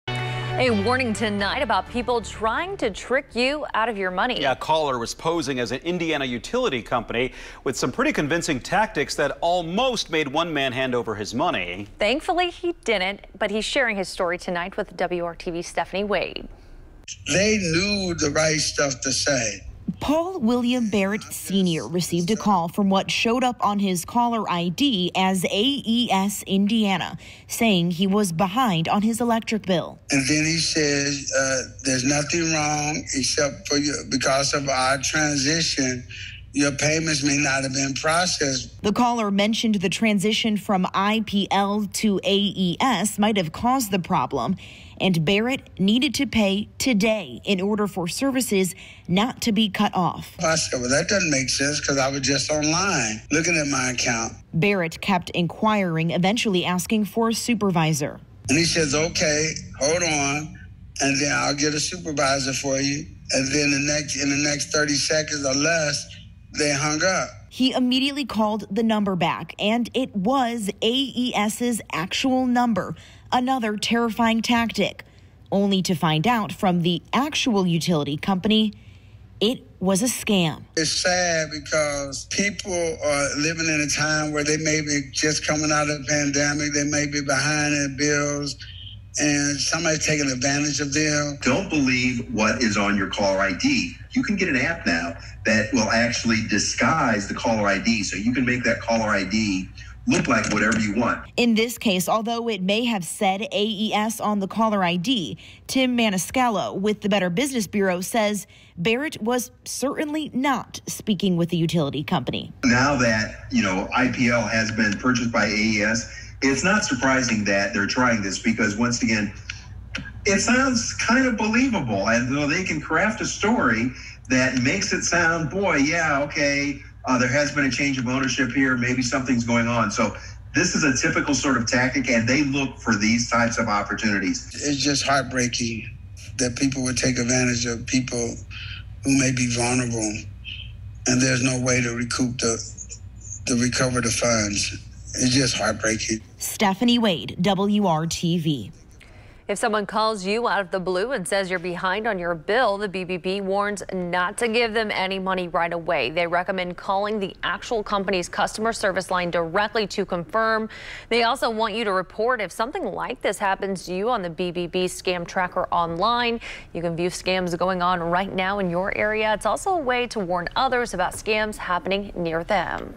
Scammer posing as utility company
Stephanie Wade talks to a man who almost fell victim to the scam.